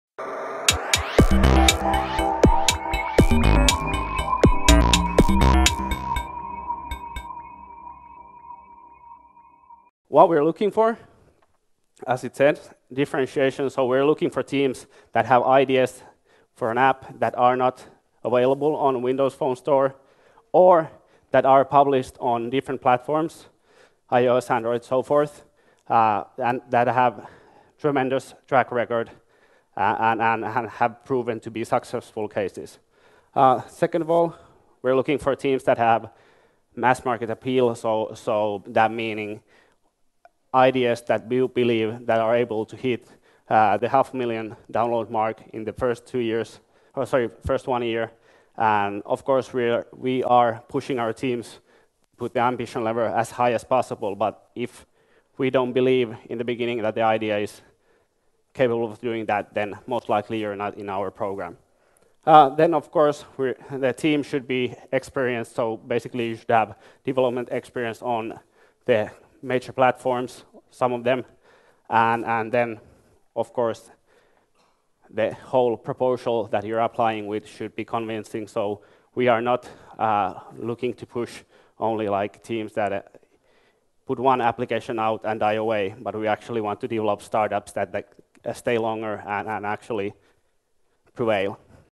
Toni Peramaki: What we are looking for? - Startup Turkey 2014
Toni Peramaki

Toni Perämäki is the Chief Operating Officer at Valohai. Additionally, Toni Perämäki has had 6 past jobs including Head of Program at Microsoft Turbopump market entry accelerator at Microsoft.

Instagram Very best of Turkey's Internet startups and Investors are gathering at Startup Turkey.
Leading event where startup founders, business angels, investors network with each other establish new business relationships.